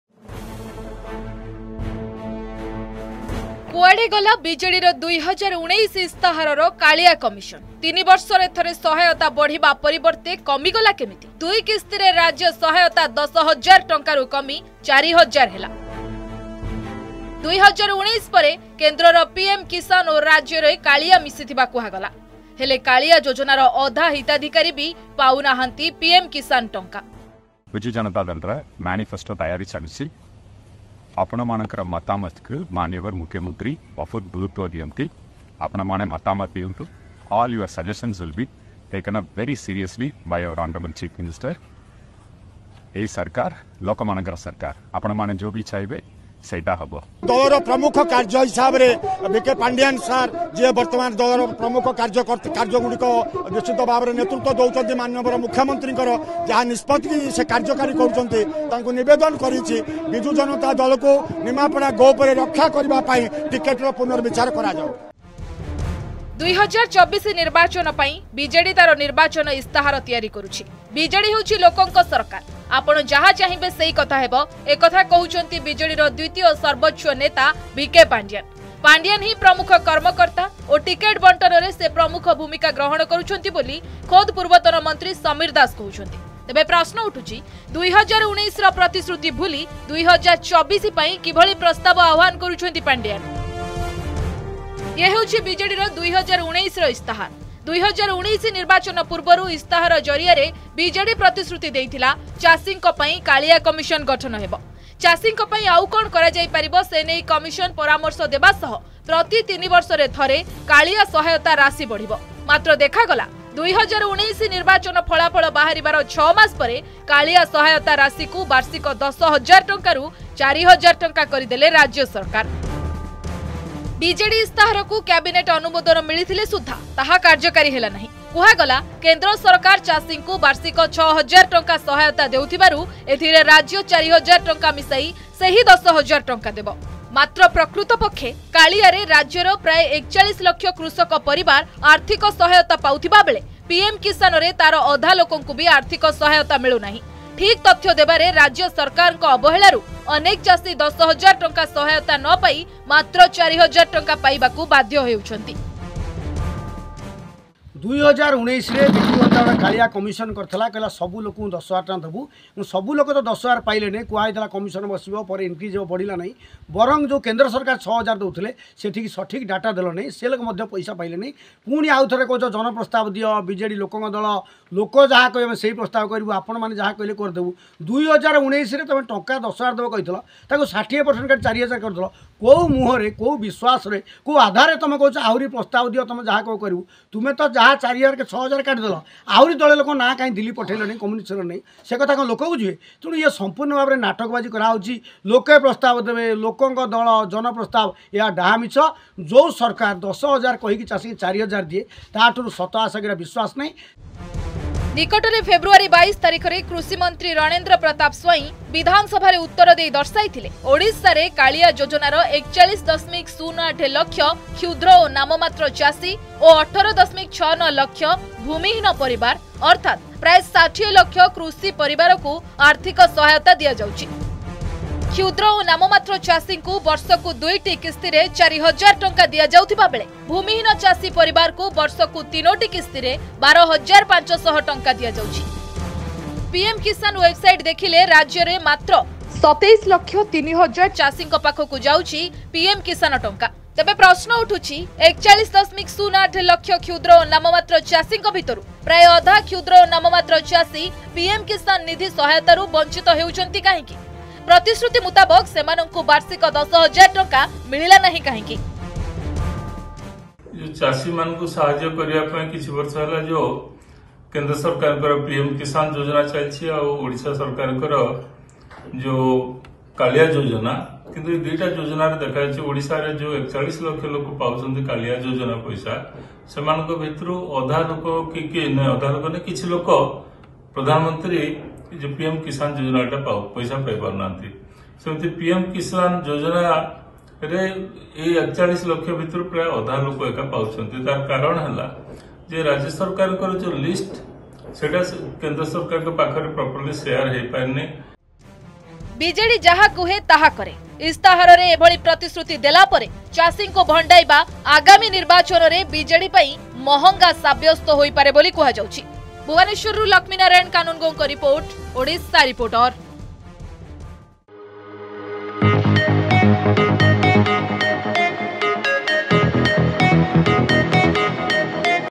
କୁଆଡ଼େ ଗଲା କାଳିଆ କମିଶନ? | Odisha Reporter
କୁଆଡ଼େ ଗଲା ବିଜେଡିର 2019 ଇସ୍ତାହାରର `କାଳିଆ କମିଶନ’। ତିନି ବର୍ଷରେ ଥରେ ସହାୟତା ବଢ଼ିବା ପରିବର୍ତ୍ତେ କମିଗଲା କେମିତି? ଦୁଇ କିସ୍ତିରେ ରାଜ୍ୟ ସହାୟତା 10 ହଜାର ଟଙ୍କାରୁ କମି 4 ହଜାର ହେଲା । 2019 ପରେ କେନ୍ଦ୍ରର ପିଏମ-କିସାନ ଓ ରାଜ୍ୟର କାଳିଆ ମିଶିଥିବା କୁହାଗଲା, ହେଲେ କାଳିଆ ଯୋଜନାର ଅଧା ହିତାଧିକାରୀ ବି ପାଉନାହାନ୍ତି ପିଏମ-କିସାନ ଟଙ୍କା।